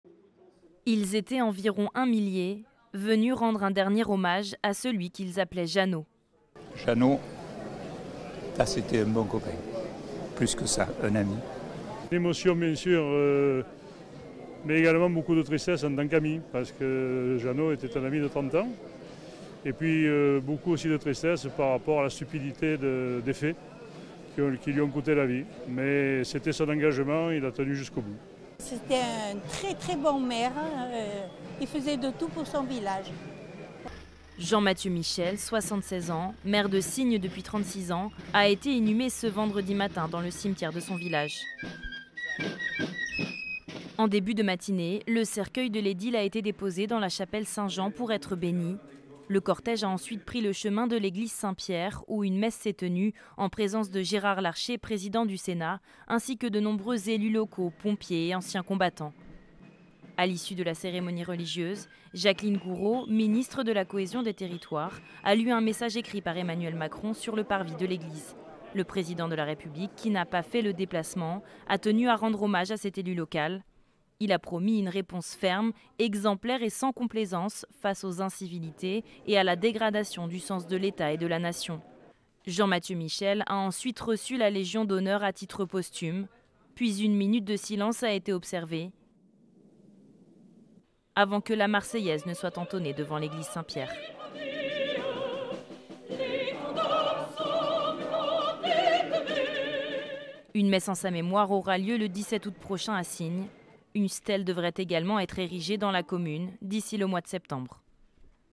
Émotion et recueillement aux obsèques du maire de Signes
Ce vendredi, la commune de Signes dans le Var a rendu un hommage à son maire, Jean-Mathieu Michel, décédé lundi renversé par un camion alors qu’il s’opposait à un dépôt de gravats sauvage.
Le président de la République Emmanuel Macron a promis une réponse «ferme, exemplaire et sans complaisance» aux incivilités et à la dégradation «du sens de l'Etat et de la nation», dans un message lu aux obsèques de Jean-Mathieu Michel.
Un sujet de Clémence Fournival et de Laura Mousset.